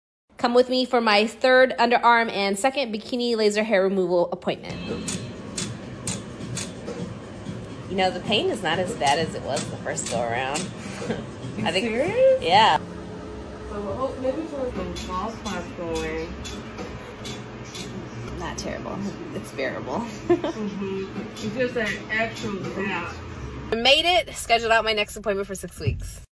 We trying to be hair free over here
We trying to be hair free over here. Laser hair removal is definitely worth the investment in my opinion